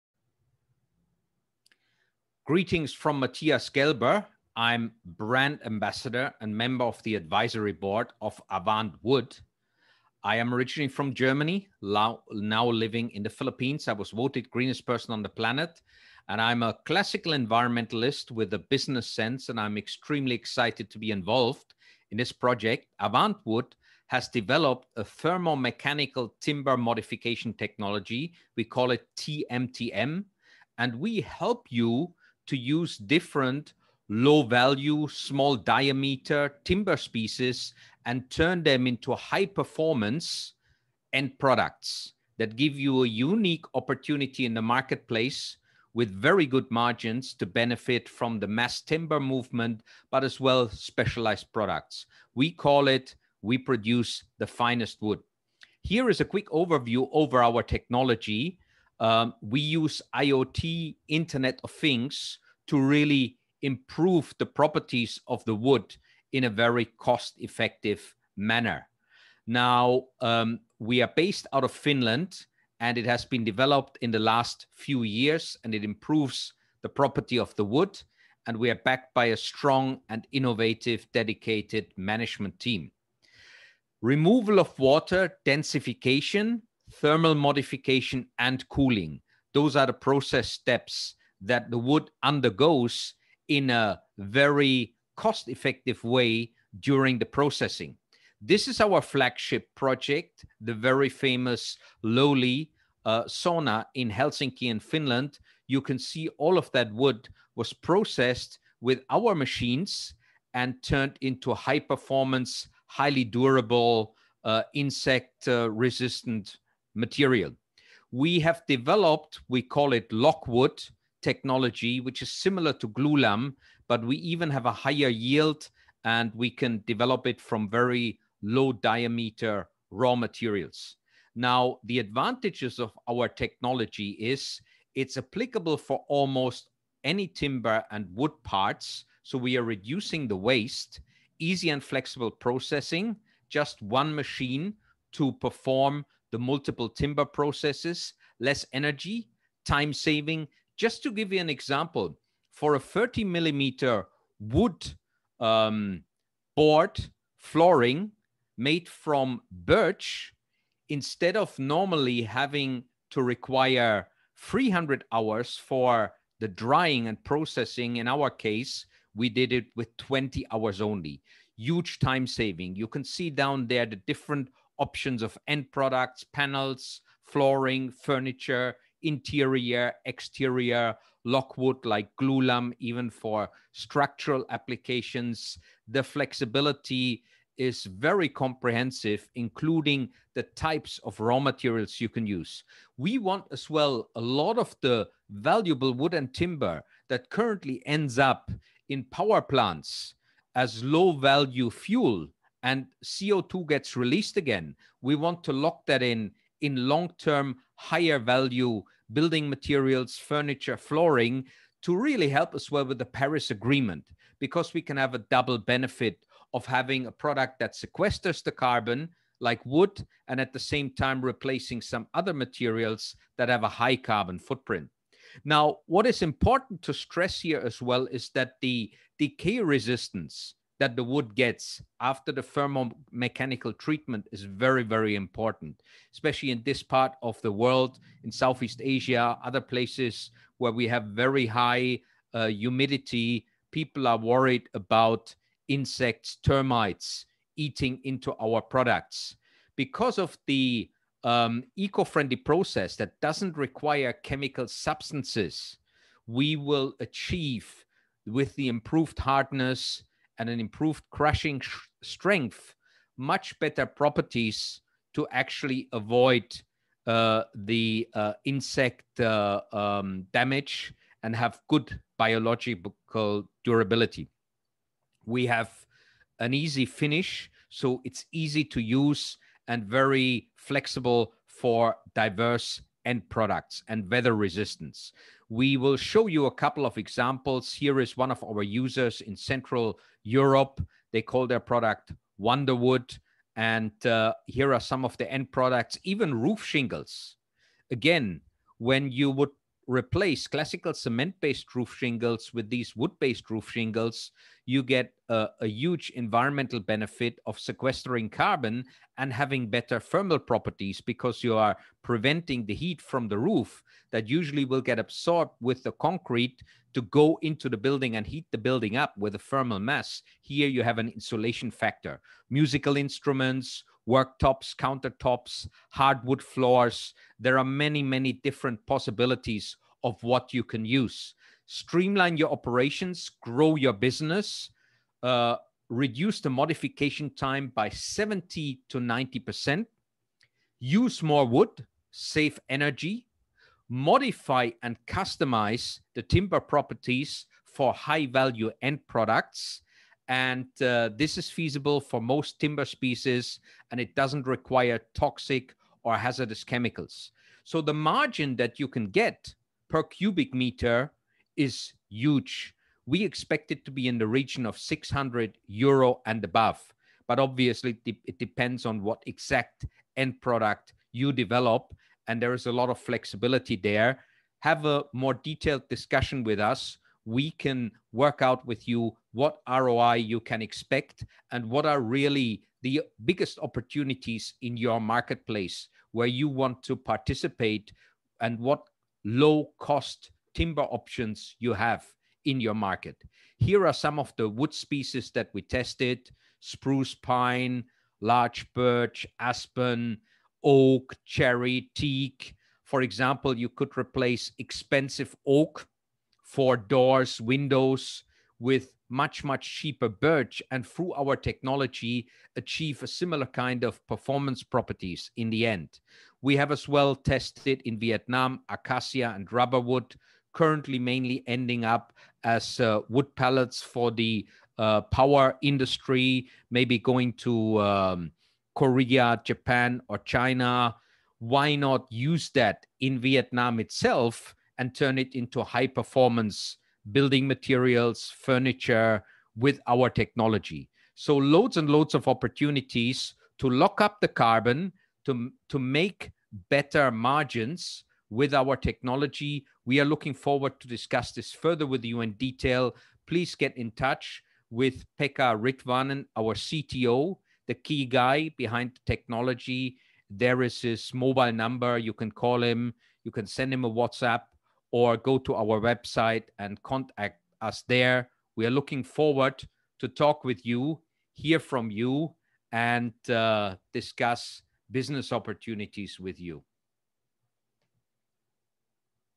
What Avant Wood's® TMTM™ technology can do with your low diameter timber resources
Too many valuable wood materials are sold with low margins as commodity products. Organizations struggle to turn low diameter wood and softwood into high-value/highly profitable products. TMTM helps to transform your raw material in a short period of time into high-performance building materials and interior products through IoT-supported thermal and mechanical transformation. The returns for your business and for the planet will expand.